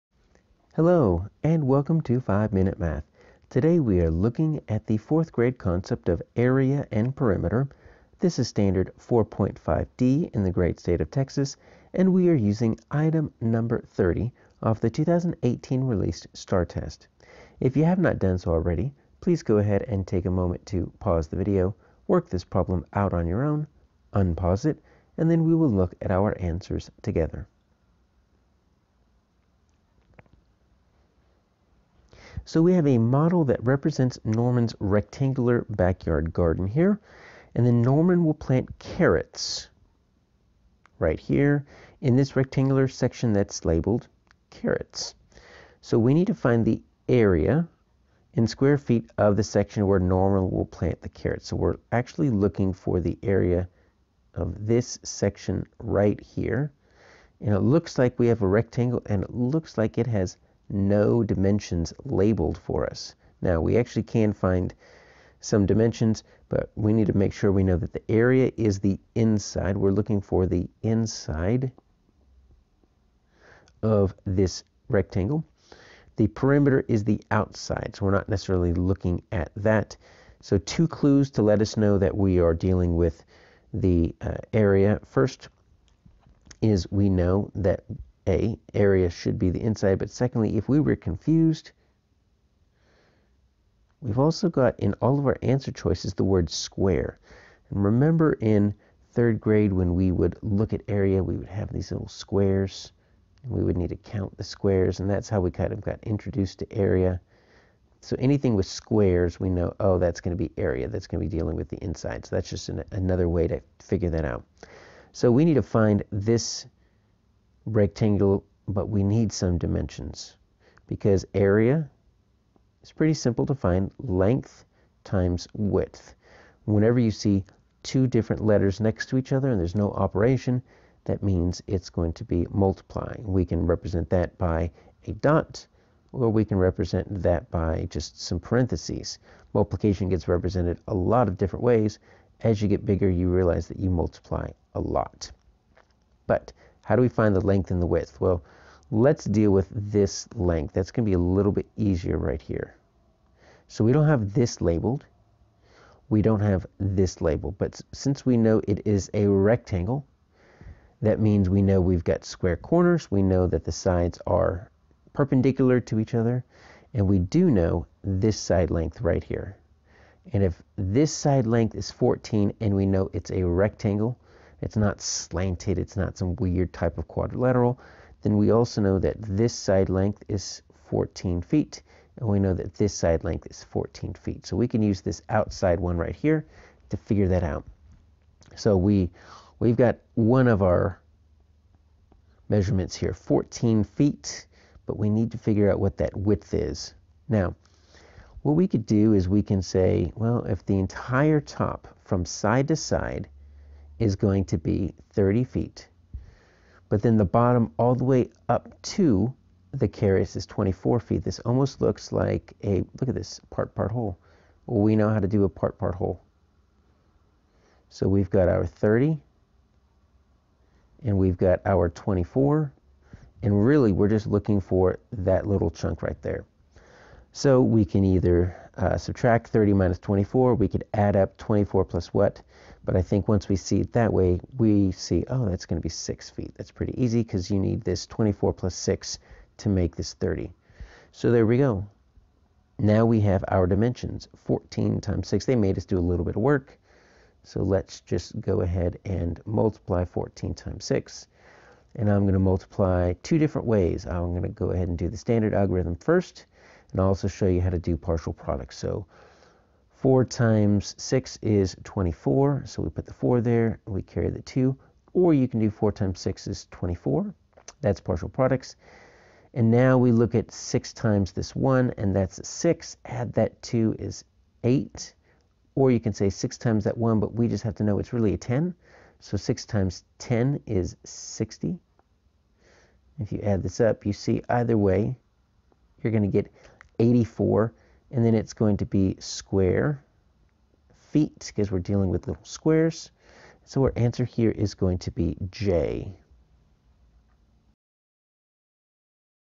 4th Grade STAAR Practice Area and Perimeter (4.5D - #7)
Practice solving problems related to perimeter and area of rectangles where dimensions are whole numbers with a test item from the 2018 released STAAR test (#30 - 4.5D).